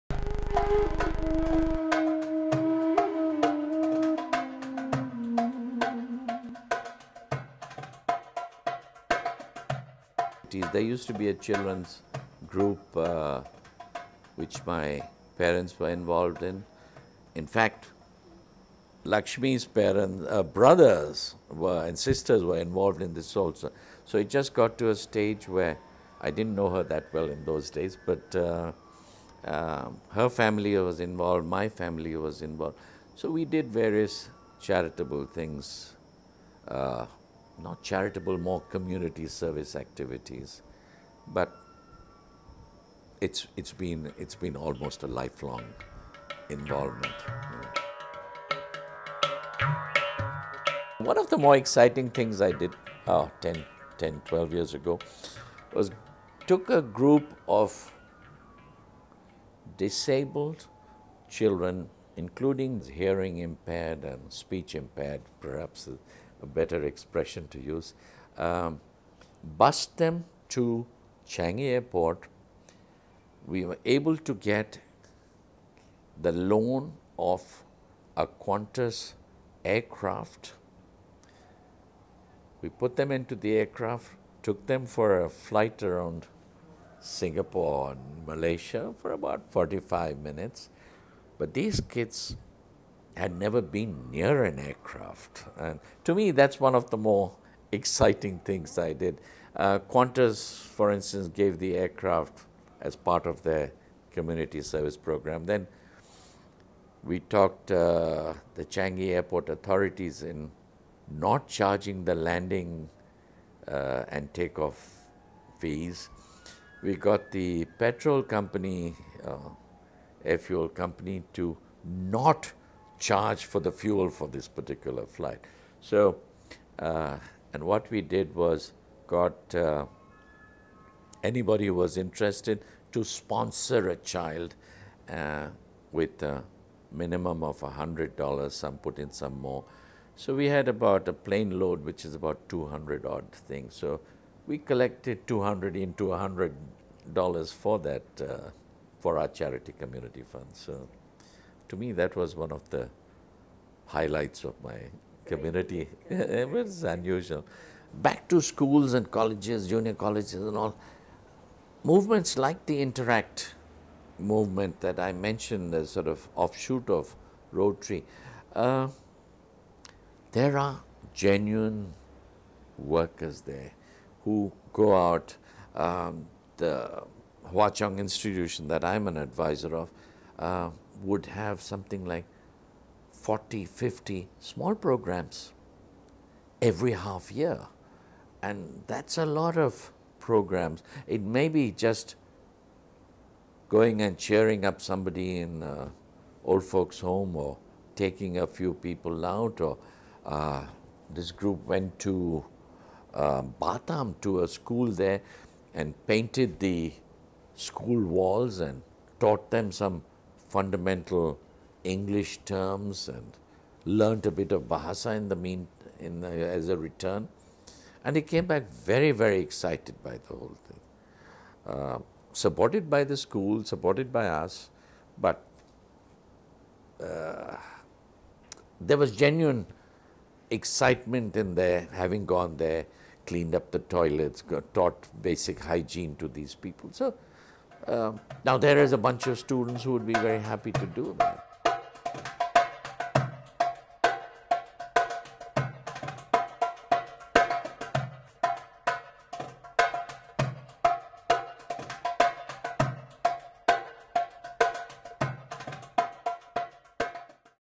UTTAM1/4 Simple Joys
Despite not starting out as a Singaporean, Mr Uttam Kripalani has definitely integrated well into Singapore and has become such a valuable contributor here. 50Faces met up with him recently to find out more about his life and various achievements. Ever the philanthropist, Mr Uttam got an early start in life mostly due to his parents who were also heavily involved in community service and would bring him along on various activities, especially those involving children.

Since those early days, Mr Uttam has come a long way and with a lifetime of volunteering to look back on, he reflects that perhaps his most fondly recalled moment would be the instance when he organised a flight for handicapped children around Singapore as part of a Rotary Club initiative. While such an initiative would not be tenable in today's aviation industry, it was certainly no easy feat to accomplish even back then. Detailing the various tasks and hoops they had to jump through to accomplish this, it is clear that this is a moment he is proud of and that the joy brought to those children definitely made it worthwhile. Despite the highs, there are also the lows and Mr Uttam mentions that one of the saddest experiences would be visiting and playing with a child one week at the Assisi Hospice, only to return the next week to find out they had succumbed to their illness. Despite this, Mr Uttam continues to organise activities such as visits to Bukit Timah Nature Reserve, Haw Par Villa and the Christmas decorations light up on Orchard Road. Simply spending time with these children and interacting with them seems to be a key message, something which can also be seen in the formation of Interact groups in many schools, a youth offshoot of the Rotary club, who are mainly community service driven and advised by senior Rotarians, one of whom is Mr Uttam.